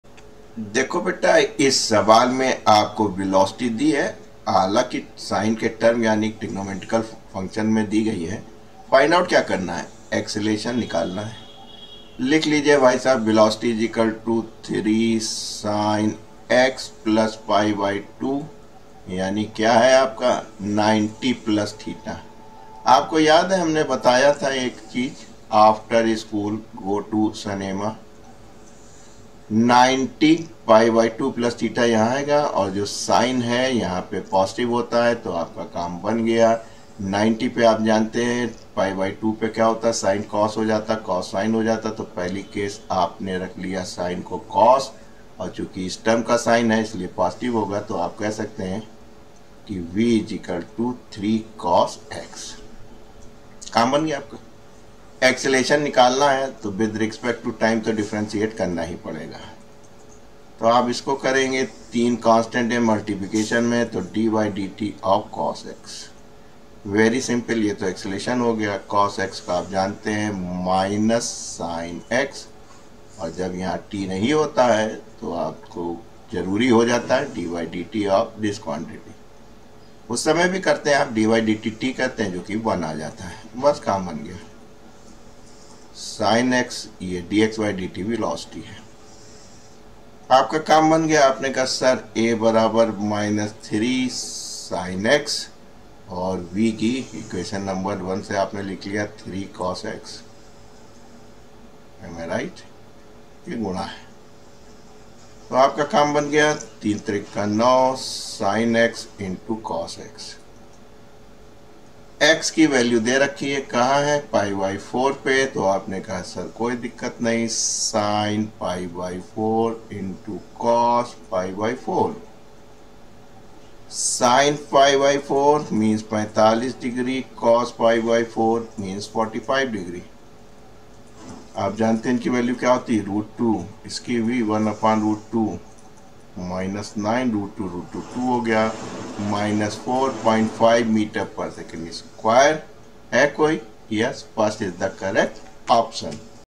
The velocity of a particle moving in straight line depends on it’s position as v = 3sin(x + π/2) m/s
The velocity of a particle moving in straight line depends on it’s position as v = 3sin(x + π/2) m/s. Find the acceleration of the particle when it is at x = π/4.